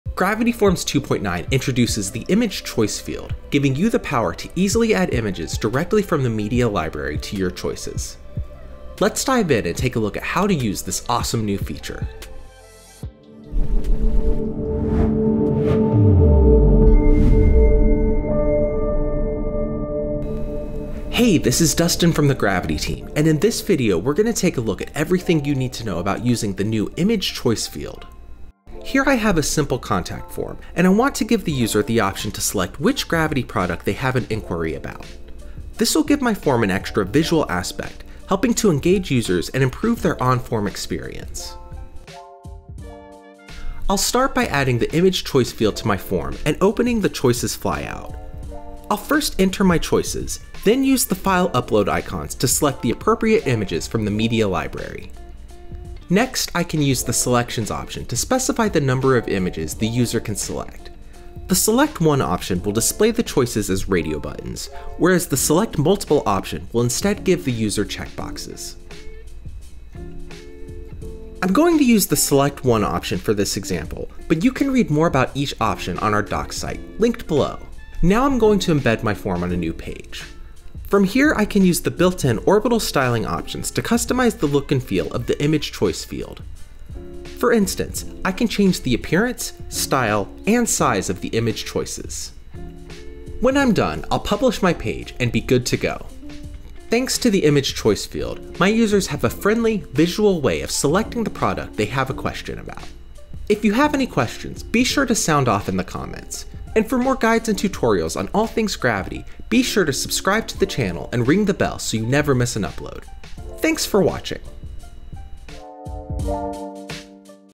Gravity Forms 2.9 Update - Using the Image Choice Field Tutorial
Gravity Forms 2.9 has just been released, and in this video, Dustin walks you through everything you need to know on using the brand new Image Choice Field. Give your forms an extra visual aspect with this stylish new field, helping to engage users and improve their on-form experience.

🖥️ To read more about the Selections option, check out our docs